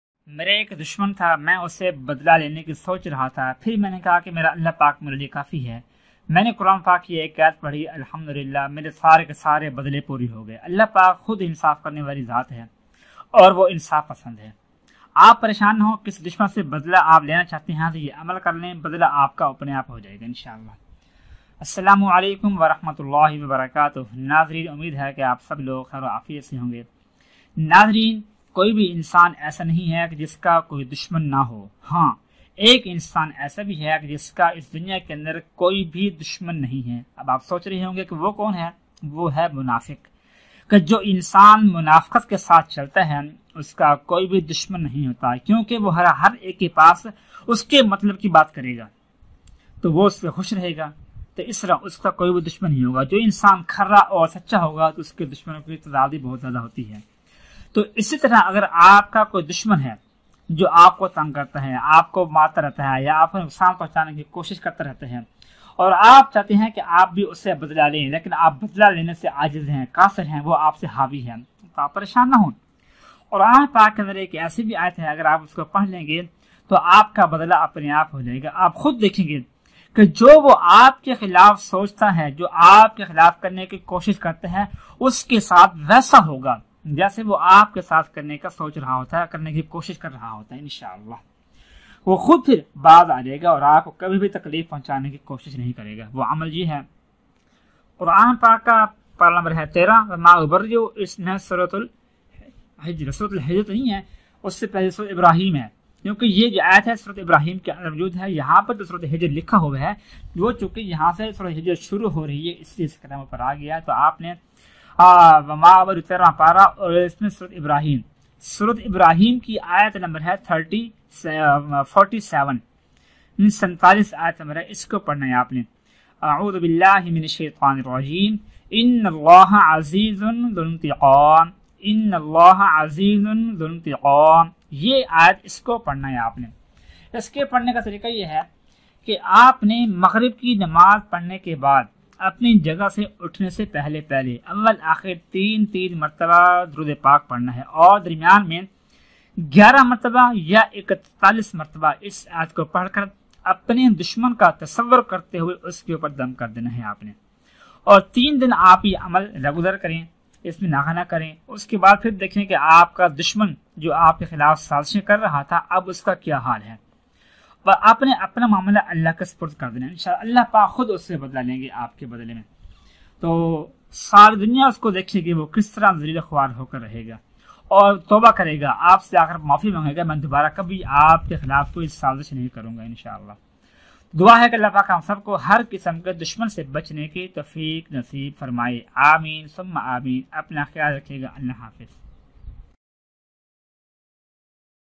Dushman se badla lene ka Powerful wazifa | Dushman se nijat pane ke Dua | Dushman se bachne ki dua
السلام علیکم؛
ناظرین اس دنیا میں کوئی بھی ایسا انسان نہیں ہے جو کہ سچا اور کھرا ہو ، اور اس کا کوئی دشمن نہ ہو ۔ بلکہ جو بھی سچا ہو گا تو اس کے دشمنوں کی تعداد بھی بہت زیداہ ہوگی۔تو اس دشمن سے اپنے آپ کو بچانے کیلئے اور اس سے اپنی حفاظت کی لئے آپ لوگ یہ عمل کریں ان شاءاللہ! دشمن آپ کا کچھ بھی نہیں بگاڑ پائےگا ۔ اور اس کےساتھ ساتھ آپ کا دشمن سے بدلہ بھی پورا ہوجائےگا۔

;;;;;;;;;;;;;;;;;;;;;;;;;;;;\_Social Links_/;;;;;;;;;;;;;;;;;;;;;;;;;;;;
You Tube Channel;